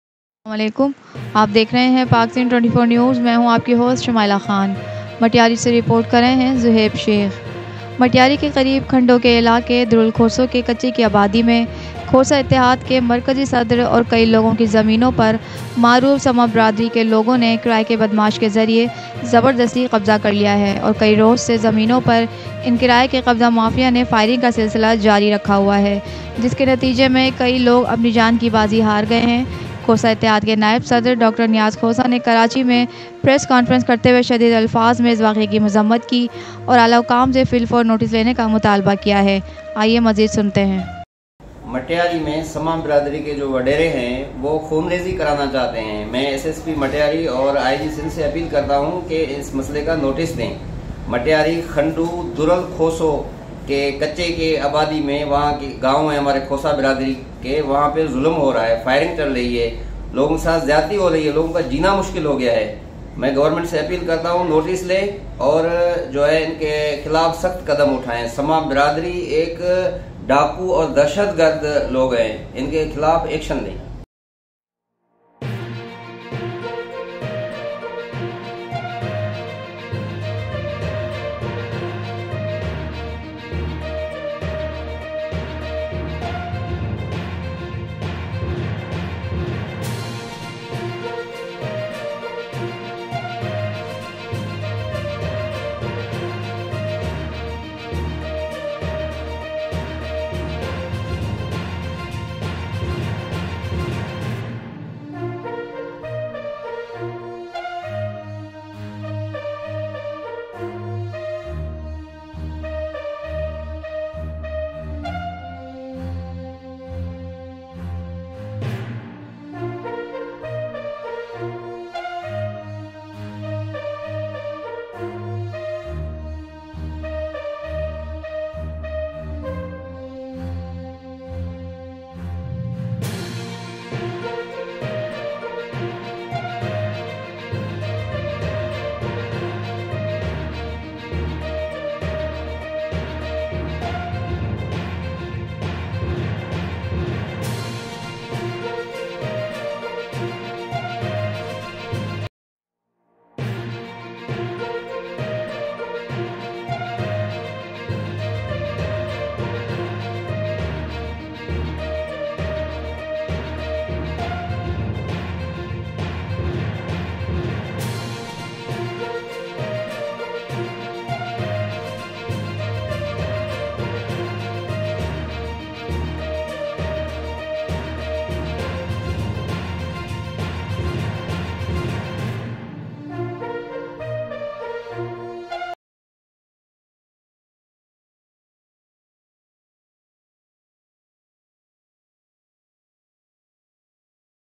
In village Darul, occupation mafia is unrestrained
In the slum of Village Darul Khoso in Khandu area near Metari, the lands of the Central President of the Khosa Alliance and several people of the prominent Saman community have been forcibly occupied by hired thugs.